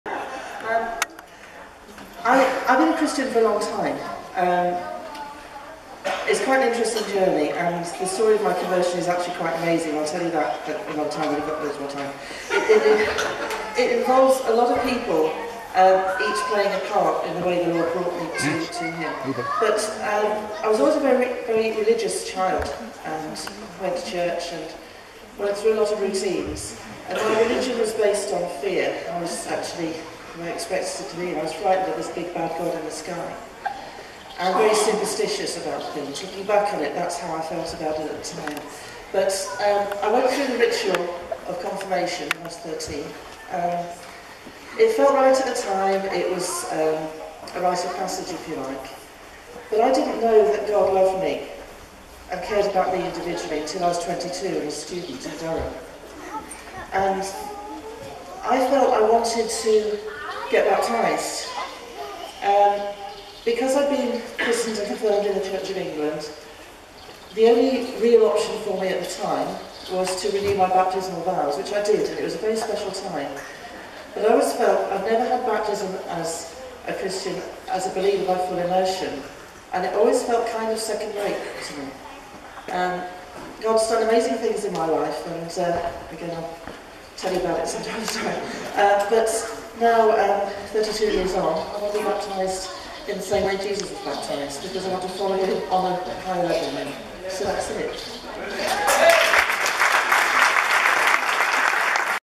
Bev Anderson's story
Bev Anderson tells part of her story ahead of being baptised at Emmanuel Church Durham on November 8, 2015.